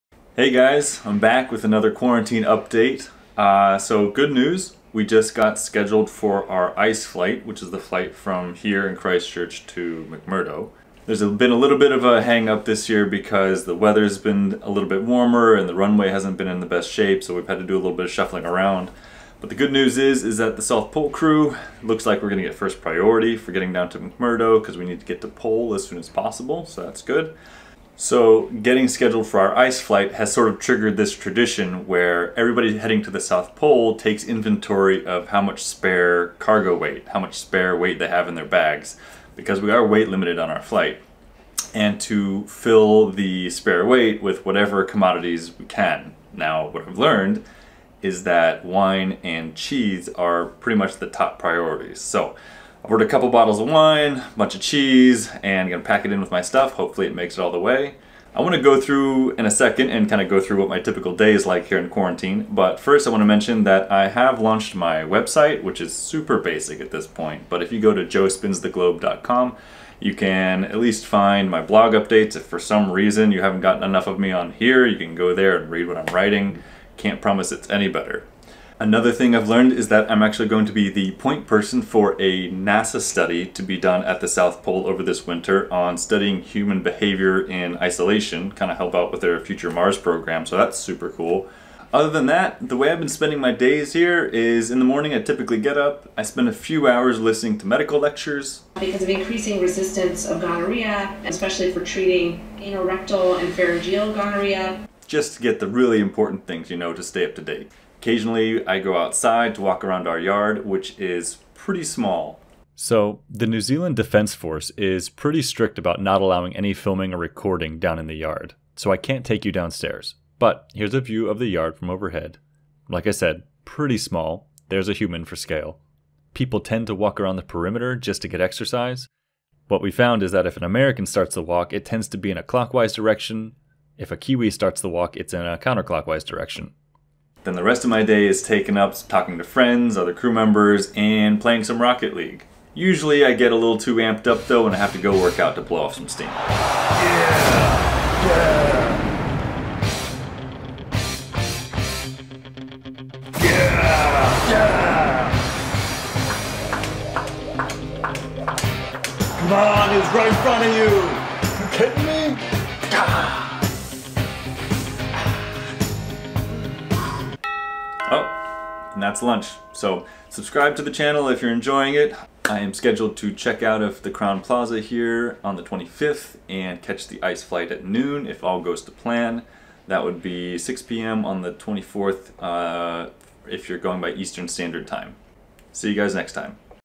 Almost done with Christchurch quarantine - South Pole vlog #2
**CORRECTION:  I've taken down the blog for now, because I've found that I'll be upload to YouTube from the South Pole!  So not as much need to write out my adventures, but it may make a return at some point.**

Eleven days into the Christchurch quarantine, and almost done... and now scheduled for the ice flight to McMurdo Station!  Also go through a typical day in quarantine and various other news.